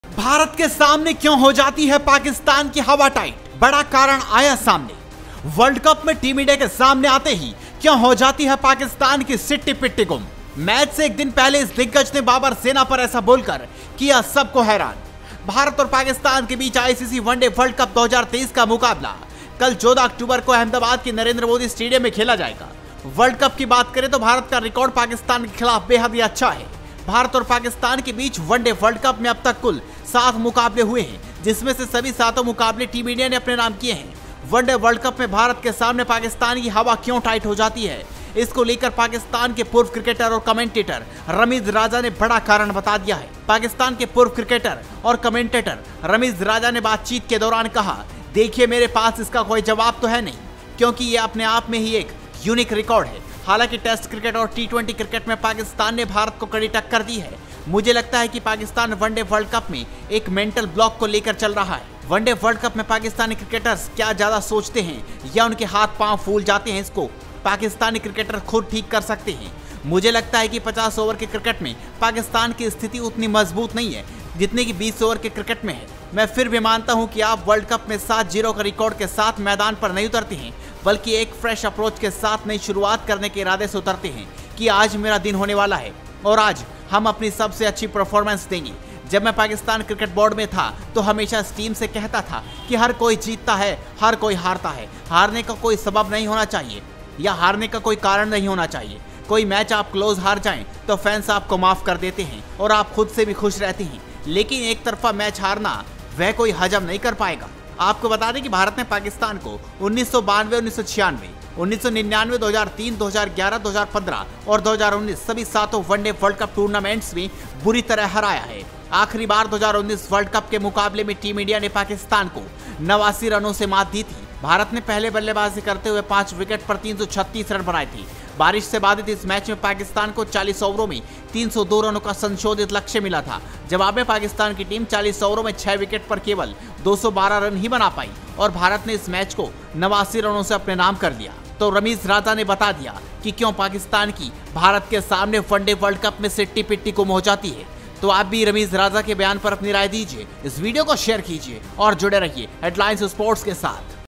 IND vs PAK: मैच से 1 दिन पहले पाक दिग्गज ने बाबर सेना पर ऐसा बोलकर किया सबको हैरान! Headlines Sports
IND vs PAK: मैच से 1 दिन पहले पाक दिग्गज ने बाबर सेना पर ऐसा बोलकर किया सबको हैरान !